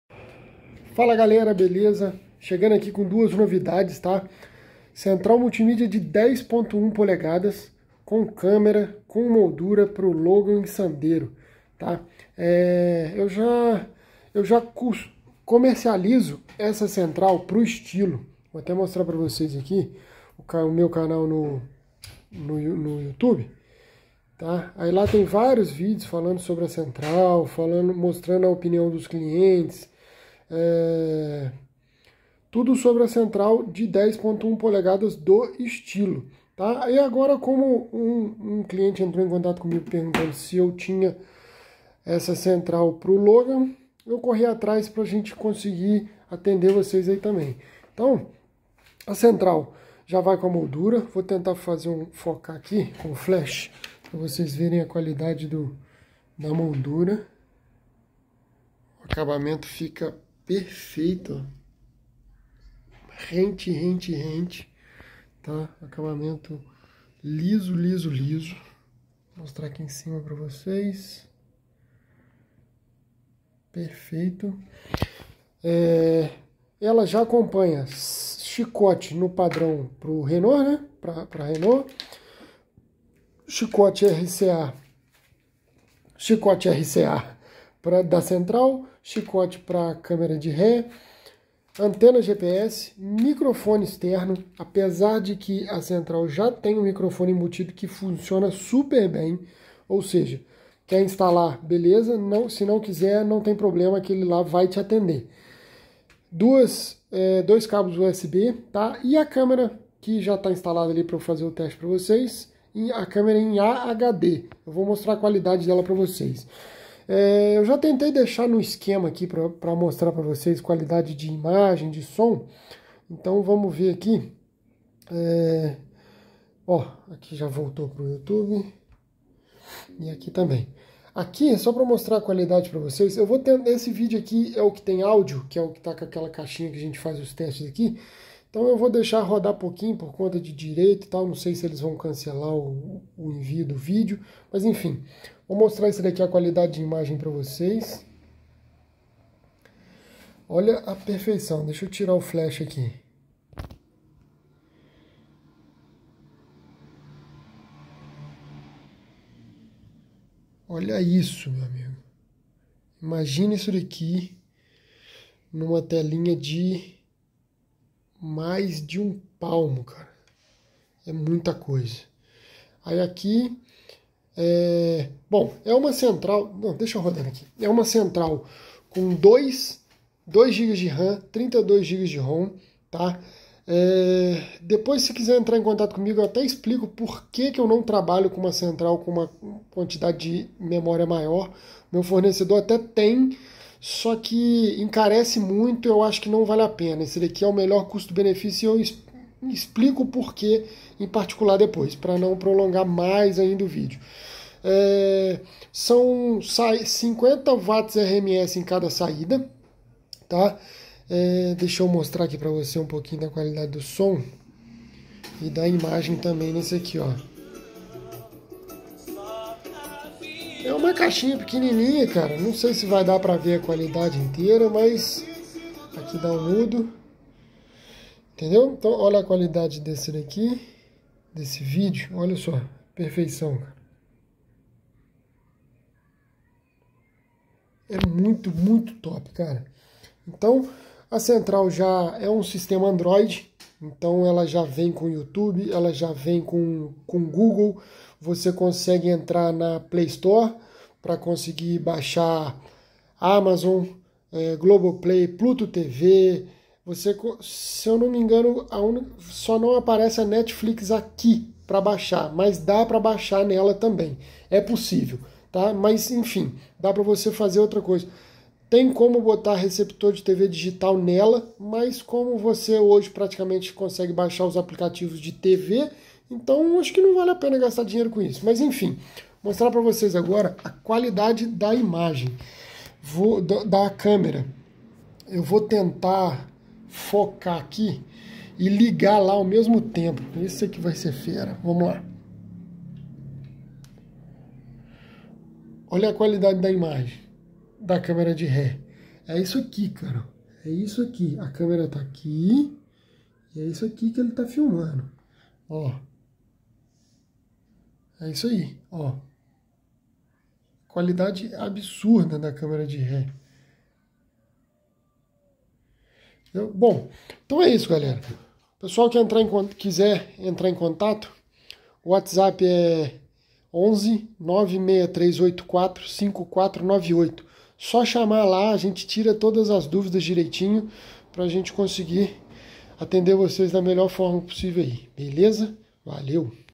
NOVIDADE NA ÁREA! AGORA TAMBÉM TEMOS A CENTRAL MULTIMÍDIA DE 10" P/ LINHA RENAULT - LOGAN / SANDERO
⚠️PRODUTO A VENDA!!

Multimídia Android com tela de 10.1" já com a moldura pra Sandero/Logan
kit composto por:

▪️Multimídia Android com tela de 10.1 polegadas
▪️Moldura
▪️Câmera de ré em AHD
▪️Chicotes e conectores Plug-in play